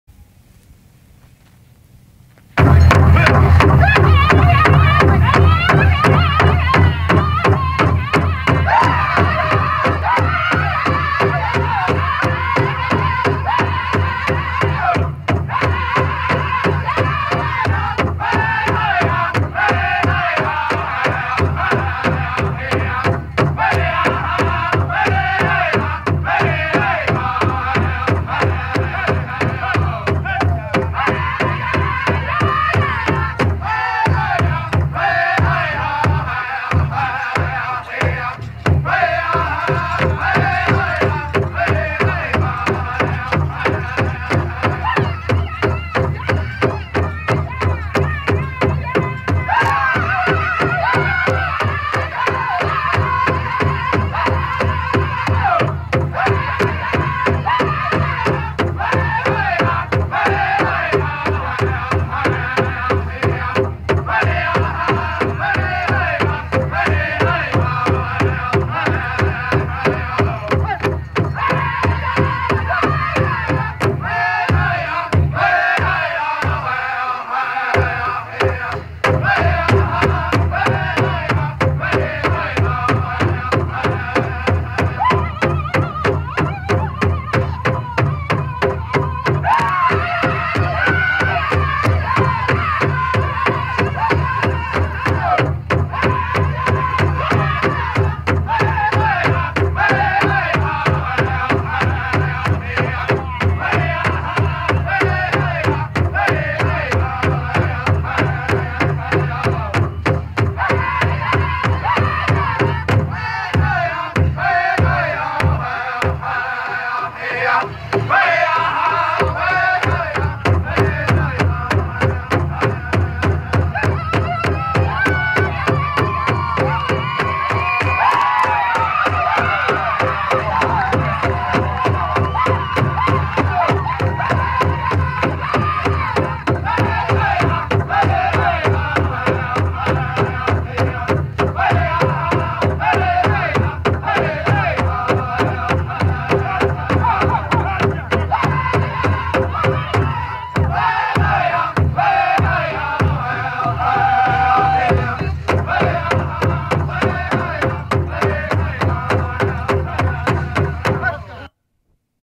Oke-tw’sha Roberts | Sacred Springs Powwow 2020 Dance Competition
Watch Oke-tw’sha perform a Fancy Shawl dance for our 2020 competition.